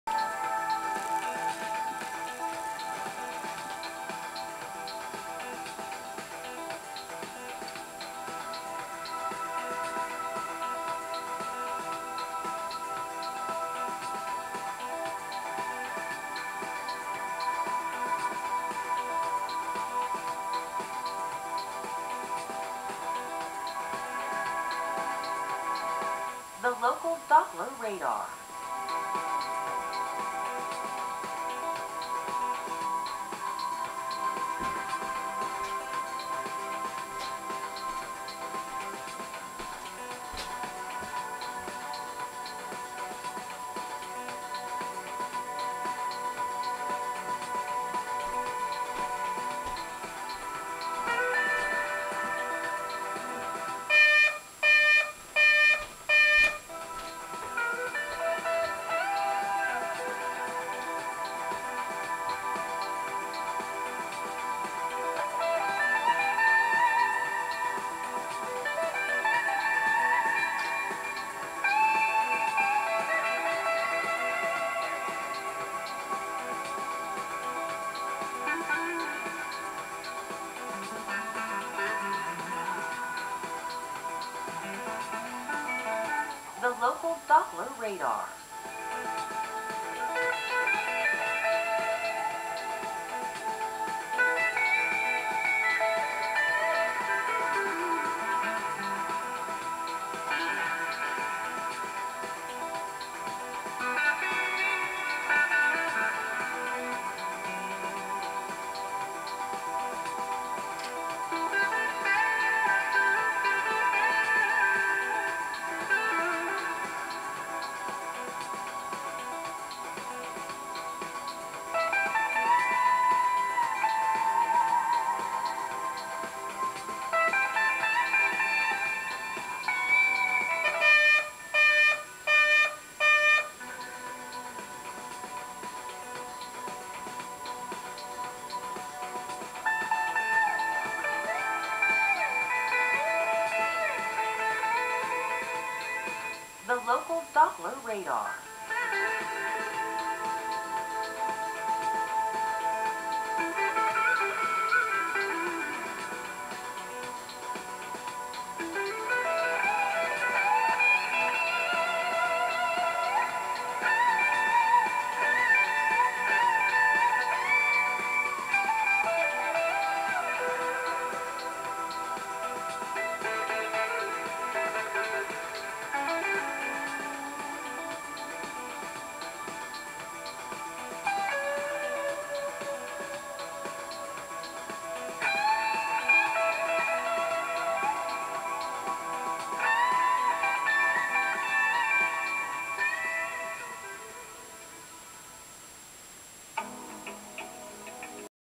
Weatherscan - Severe thunderstorm warning in Maryland - June 13, 2013
This was the second round of severe weather we had on this day. The first was in the morning as I was eating breakfast, while this one was in the afternoon.